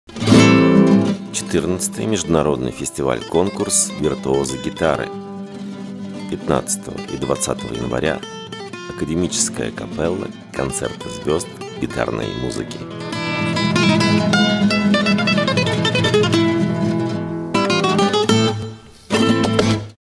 XIV Международный фестиваль-конкурс "ВИРТУОЗЫ ГИТАРЫ" 2017.
ФОРУМ гитаристов.   XIV Международный фестиваль-конкурс  "ВИРТУОЗЫ ГИТАРЫ" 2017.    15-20 января 2017 года

Миро СИМИЧ (Швеция), Оскар де ГУЗМАН (Испания), Надя БОРИСЛОВА (Мексика), Виктор КОЗЛОВ (Челябинск), Александр ВИНИЦКИЙ (Москва), Евгений БАЕВ (Тверь), Леонид КАРПОВ, Константин ИЛЬГИН, Дмитрий ВОРОНИН, Юрий БОБЫЛЕВ (Санкт-Петербург) и многие другие